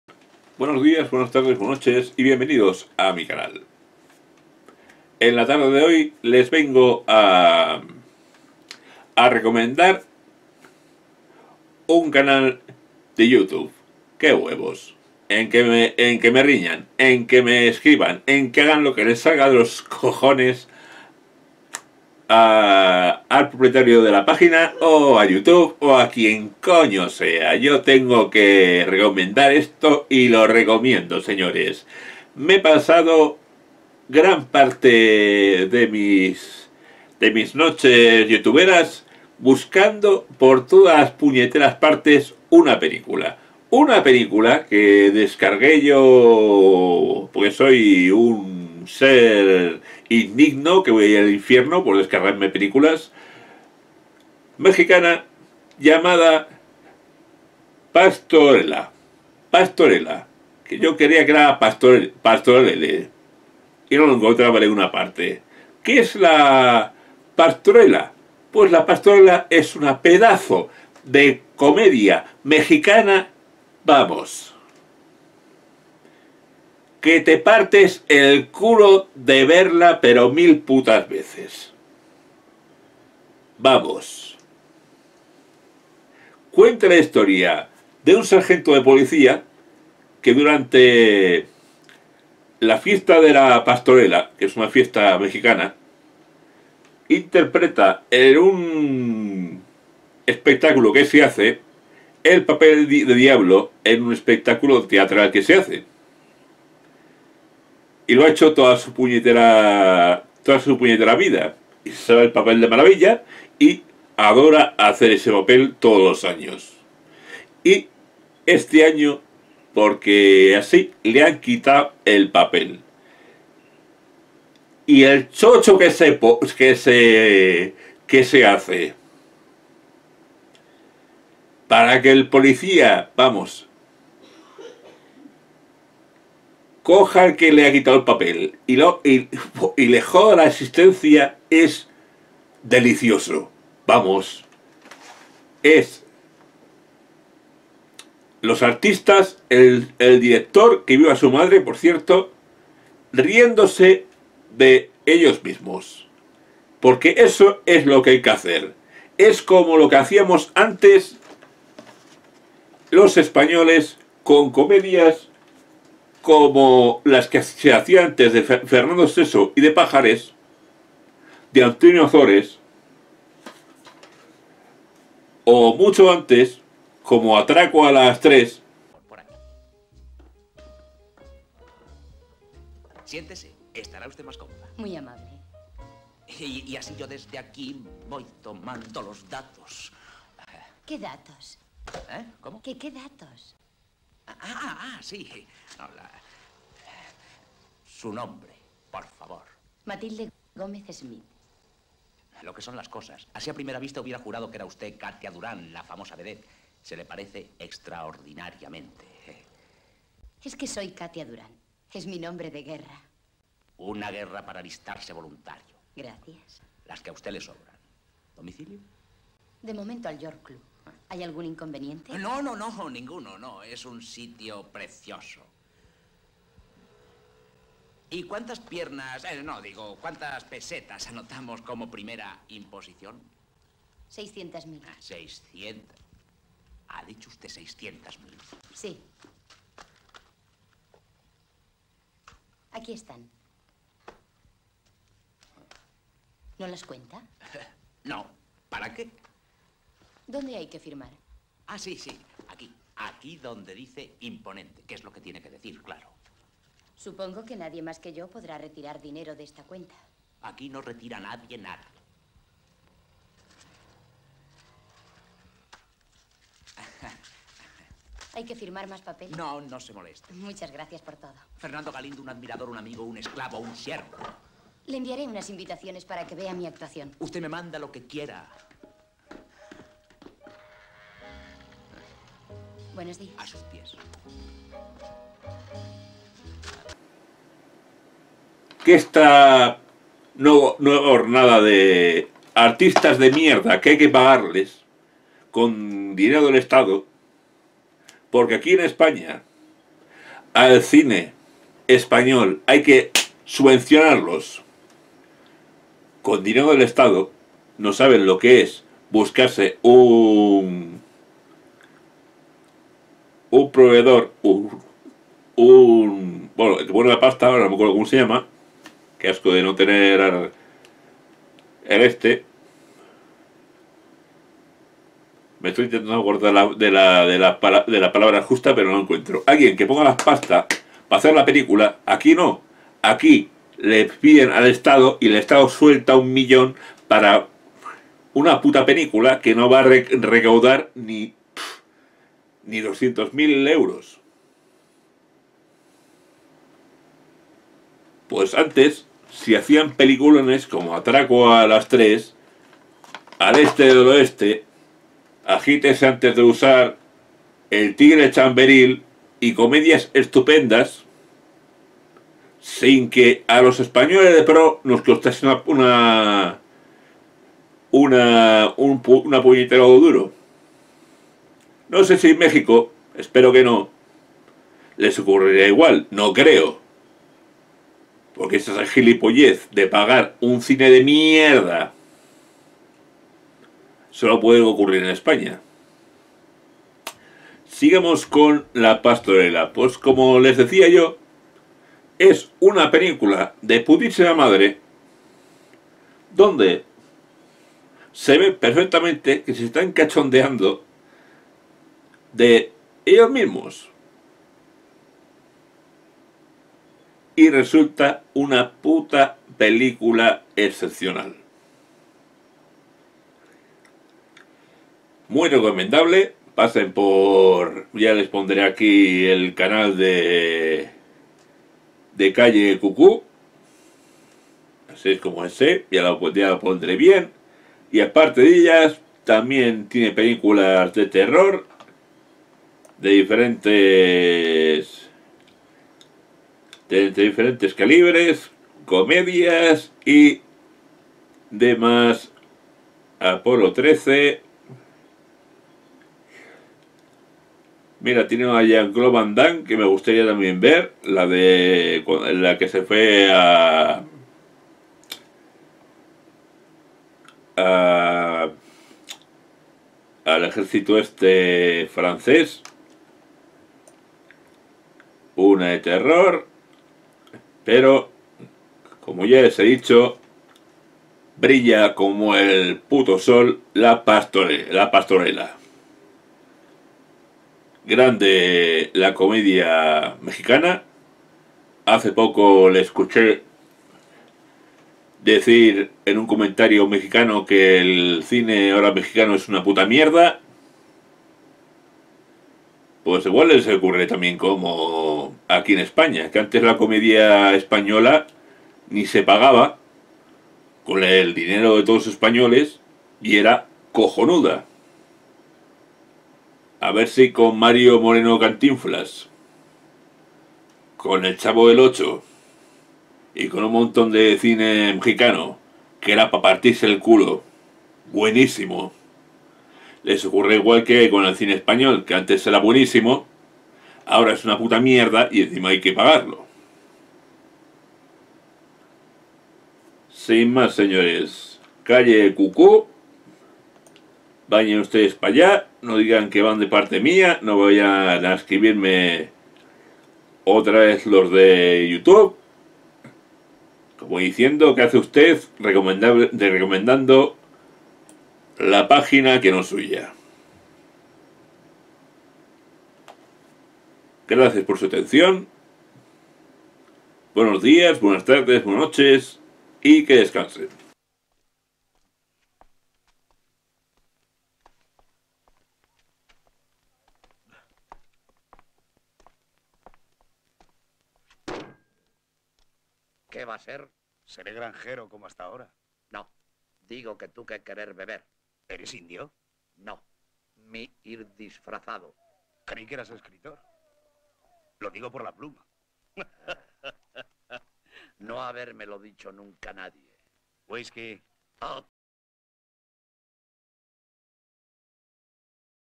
RECOMIENDOPELICULAS GRATIS EN YOUTUBE: PASTORELA
Peliculón desde Mexico  de la mano de calleb qqq  una comedia cojonuda y un canal lleno de posibilidades Y AHORA QUE SE QUEJEN